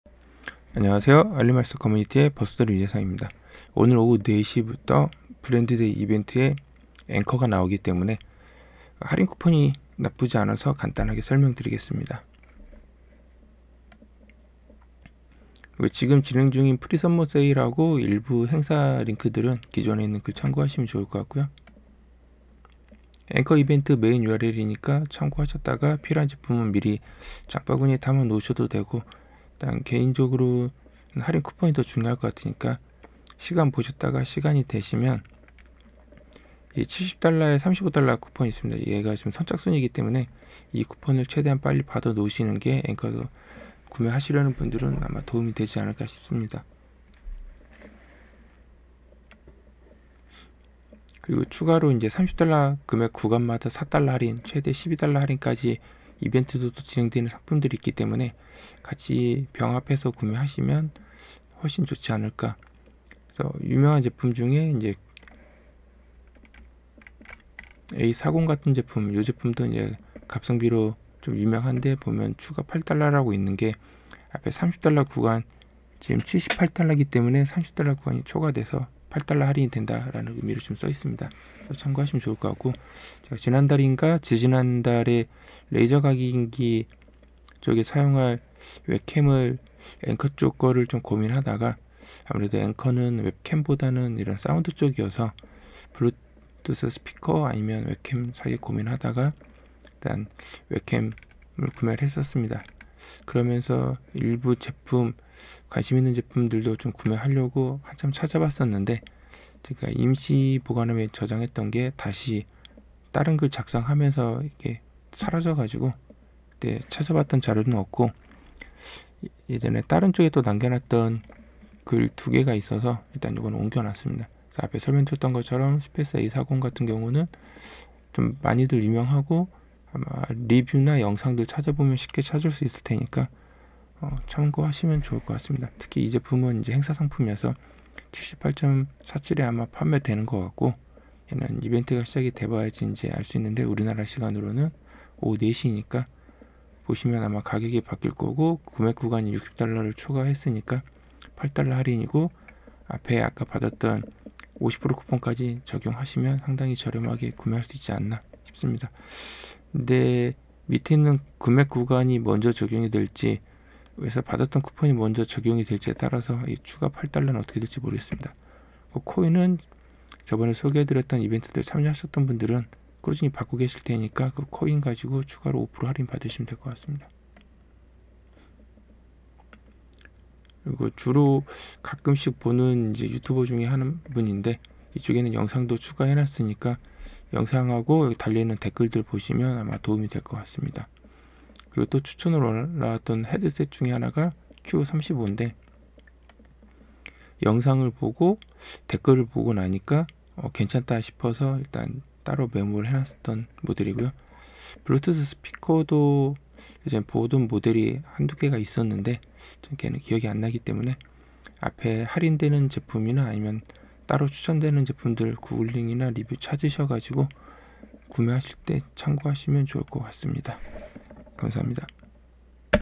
[버섯] Aliexpress Anker 브랜드 데이 이벤트(최대 선착순 50% 쿠폰)
안녕하세요 알리마스터 커뮤니티의 버섯돌이 유재성입니다.
오늘 오후 4시부터 알리익스프레스 브랜드데이에 ANKER 제품이 나옵니다.
선착순으로 제공되는 $70이상 $35 할인 쿠폰이 괜찮아 보여서 급하게 포스팅과 영상을 준비했으니 모쪼록 알뜰하게 구매하시는데 조금이라도 도움이 되기를 희망합니다.^^

Anker 이벤트
Anker A40

화면 녹화 마이크 : TITAN PRO